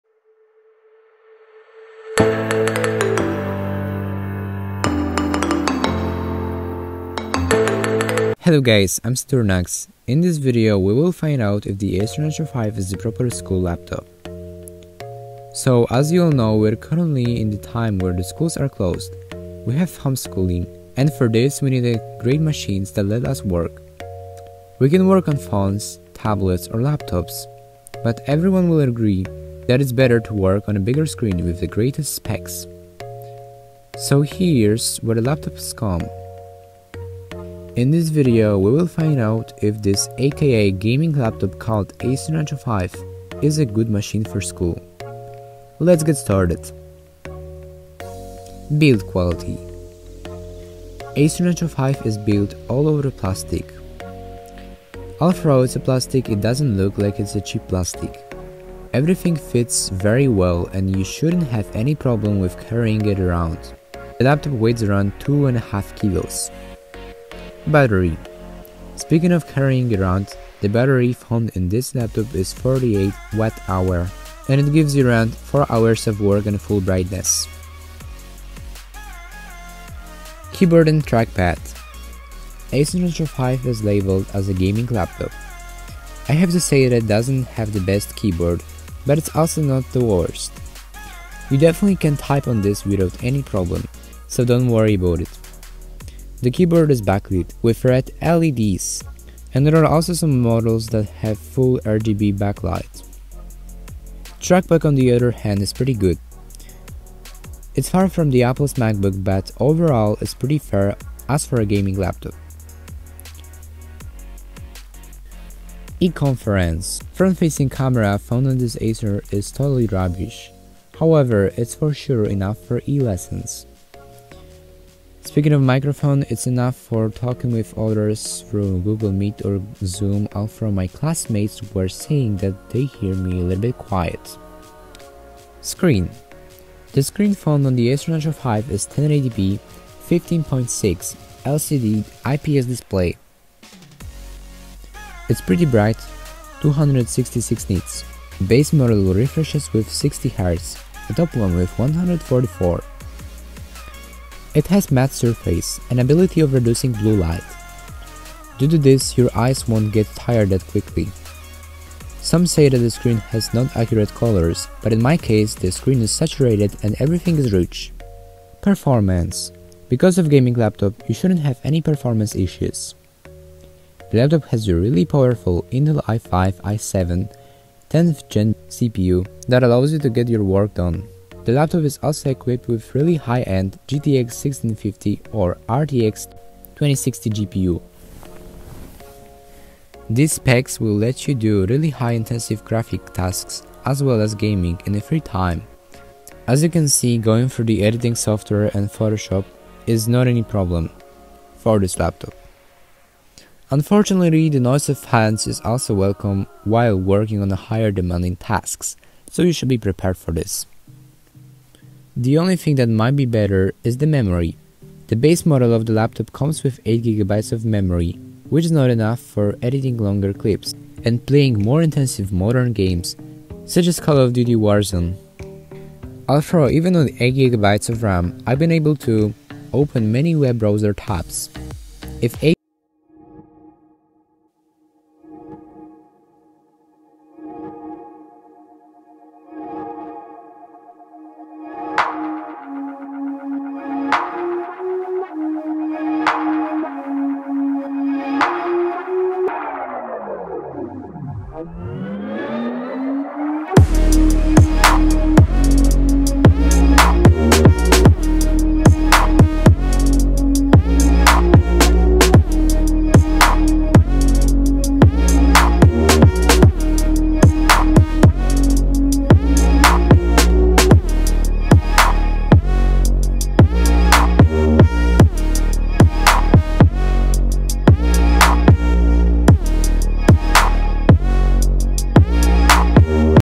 ACER NITRO 5 | BEST LAPTOP FOR EDUCATION?! (2020)
Hello guys

In this video I’m testing laptop by the name of Acer nitro 5. A lot of people were asking me if the laptop is good for school work. My answer is yes and no. If you’re looking for a laptop that is light and you don’t need the best specs Acer nitro 5 won’t be the best choice. But if you are for example a graphic or video editor, and you need an additional performance the laptop is just the best option for you. You won’t find any better machine for this price.

Watch the video and find out why!

BUY LAPTOP HERE👇👇👇

GTX 1650 MODEL

RTX 2060 MODEL

Video section:

Intro:(0:48)
Build quality:(1:07)
Battery:(1:21)
Keyboard/trackpad:(1:58)
E-conference:(2:15)
Screen:(2:48)
Performance:(4:12)
Price:(4:20)
Outro:(4:43)